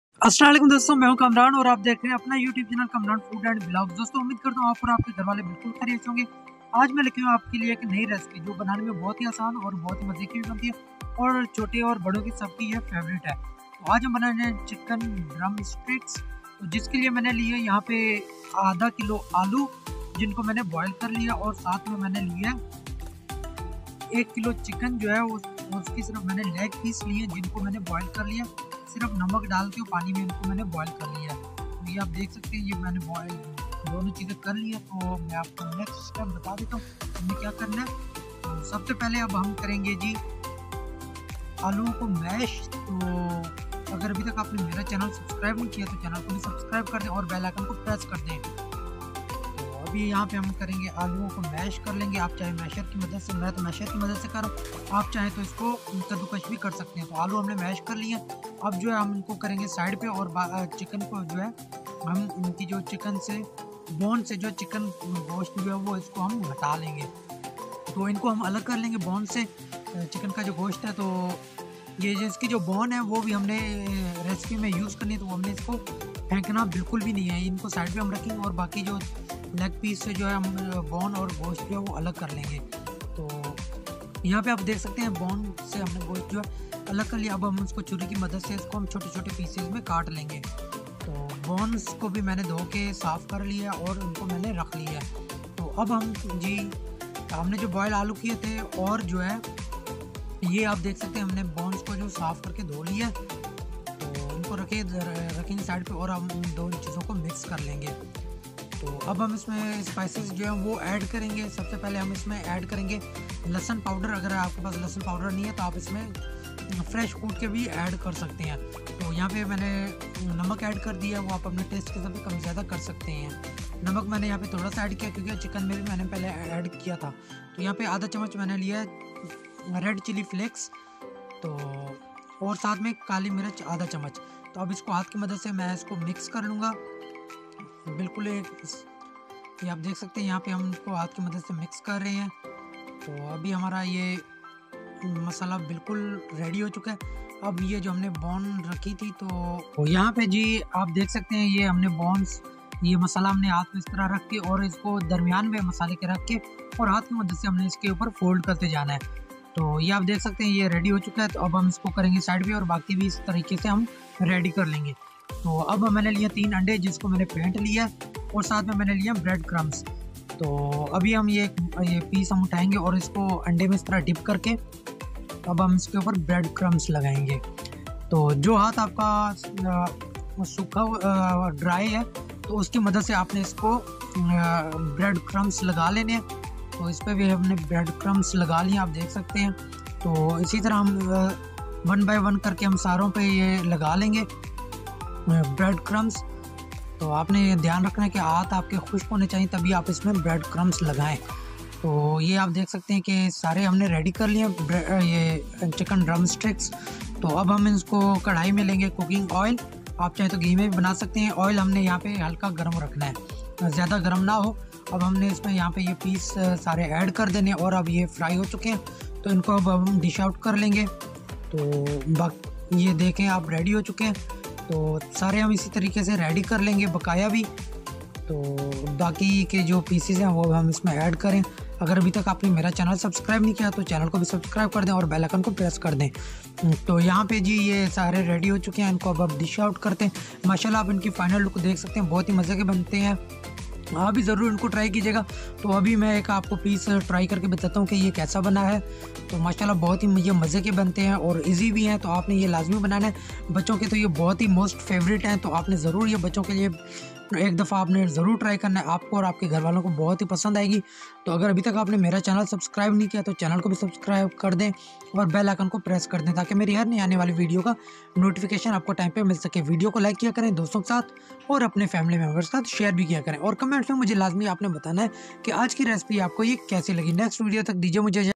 Bakery Style Chicken Drumsticks Recipe - Homemade Chicken Drumsticks Recipe - Kamran Food & Vlogs
Aj me ap k lye laya hun chicken Drumsticks recipe. Drumsticks recipe. fried chicken recipe. crispy fried chicken recipe by kamran food & vlogs. ijaz Ansari recipes.